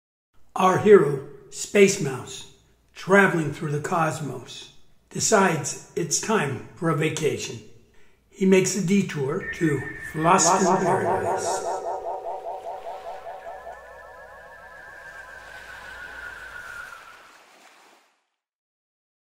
logo and island Final
Another exciting adventure of Space_Mouse.
Space needs a break so he detours to Floston Paradise...